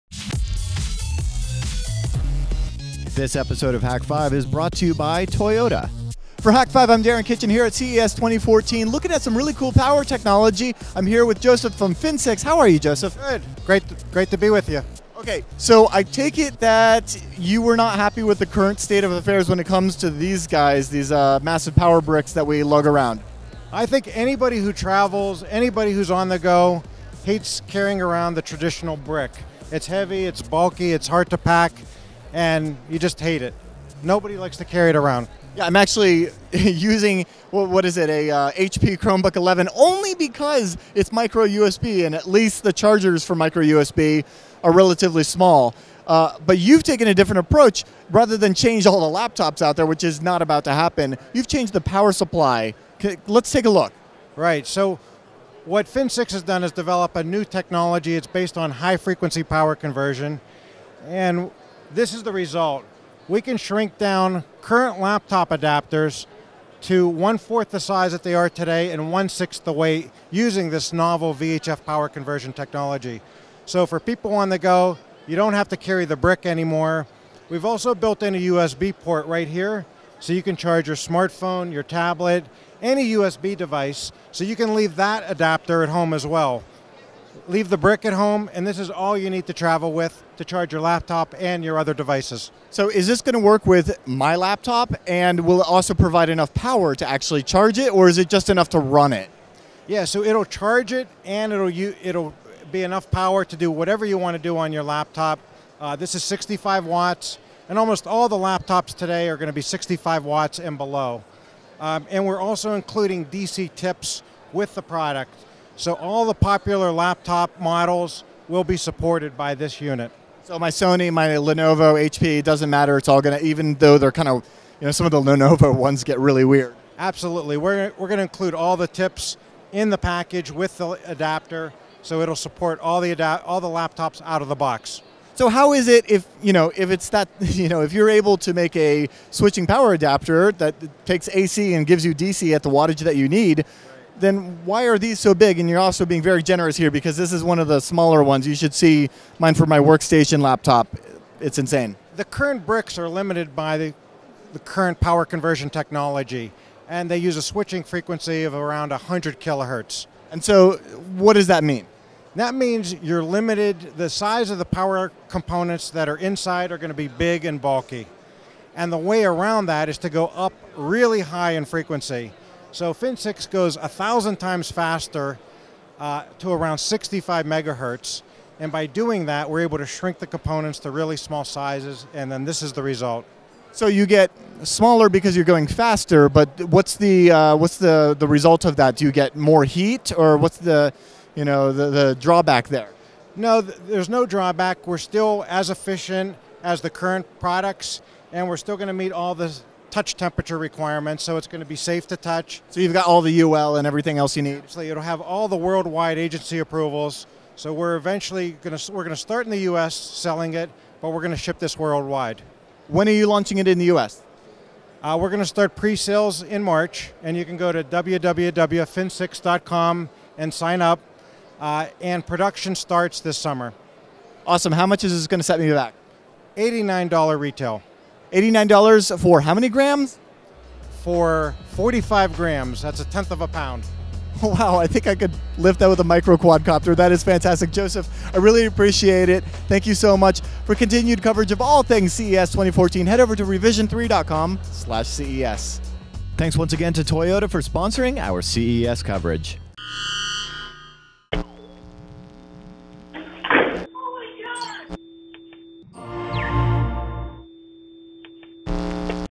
Finsix - The Small Lightweight Laptop Power Supply
Hak5 -- Cyber Security Education, Inspiration, News & Community since 2005:
Finsix is a power supply that has been shrunk down and lost a bit of weight that is available for all you laptop power needs.